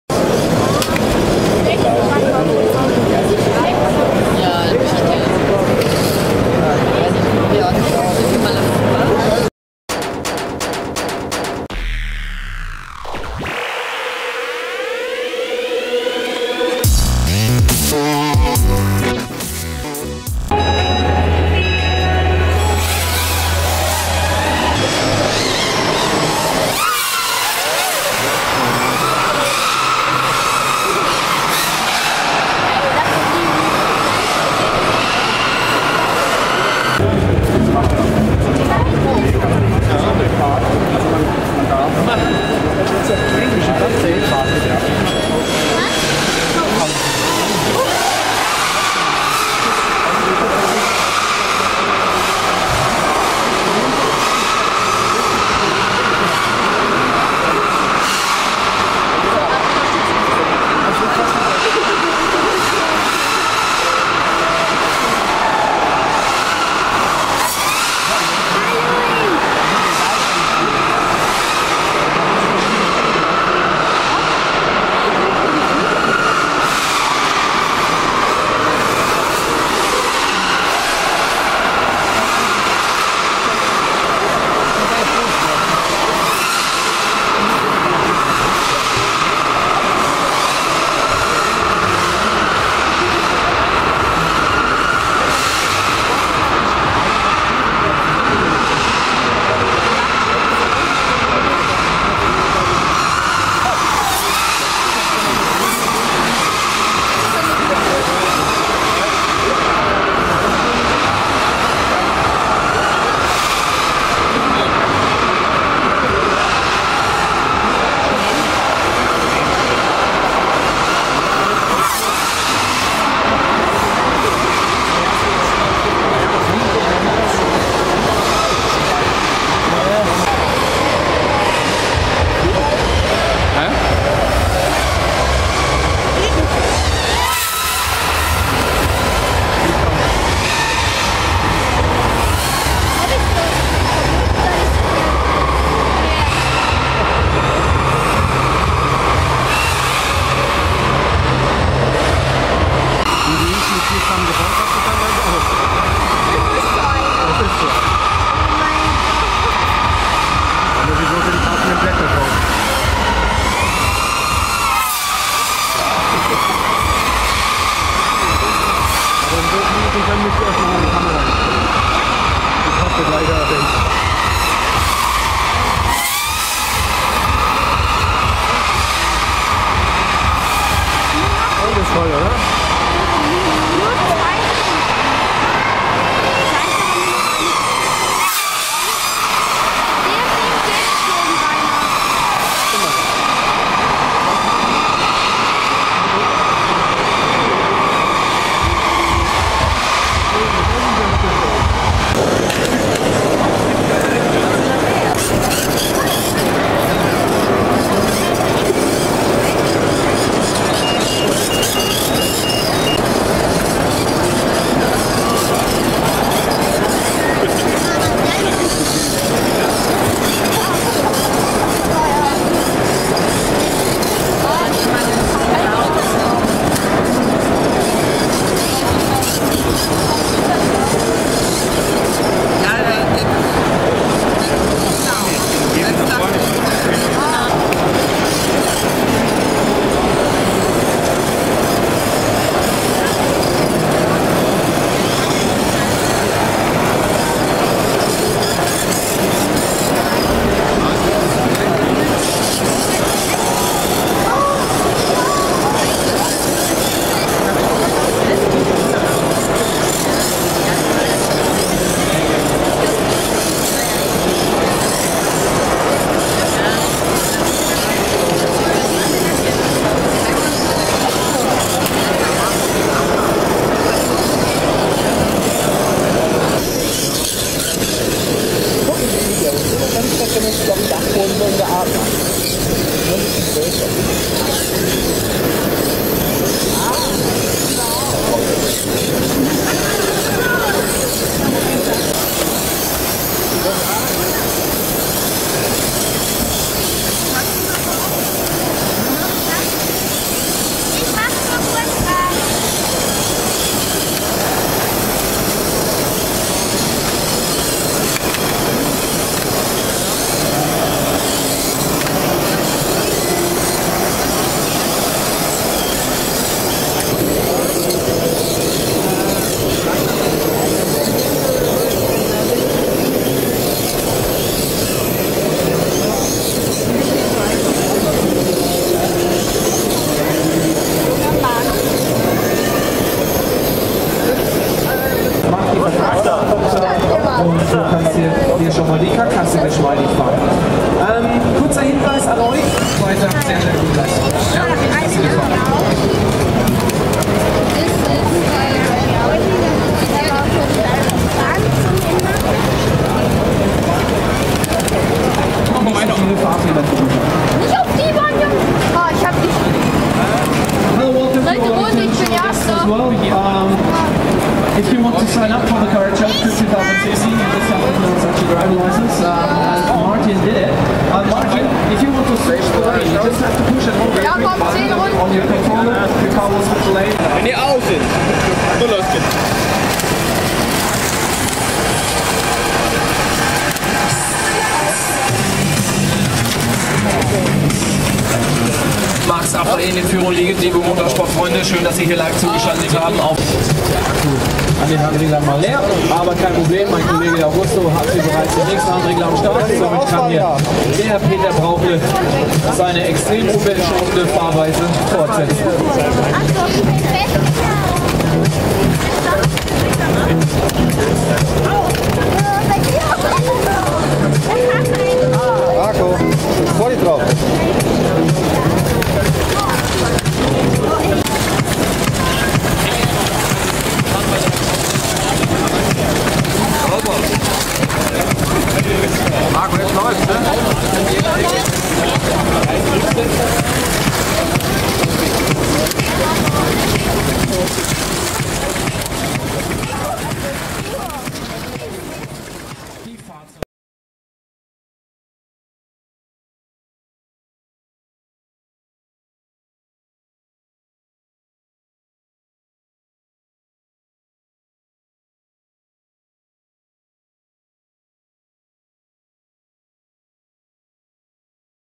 Faszination Modellbau 2016 - Neuheiten!
Hallo Modellbaufans -

es war wieder Zeit für die diesjährige Faszination Modellbau in Friedrichshafen.

Ich habe euch die diesjährigen Neuheiten der Messe zusammengefasst - mit Neuheiten meine ich nicht die neuen Modelle etc. sondern neue Attraktionen, mit denen die Besucher unterhalten worden sind. :)

Den kompletten Messeumfang habe ich bereits in meinen vorherigen Videos veröffentlicht - etliche Videos über Truck Modellbau, Flugshow, Crawler, Eisenbahnen, Schiffe, Motorräder und und und...

Heute mal komplett ohne Hintergrundmusik.. da ich finde, dass mit dem "angenehmen Hintergrundrauschen einer Messehalle" das tatsächliche Feeling erst so richtig rüber kommt :D :D

rc-check.de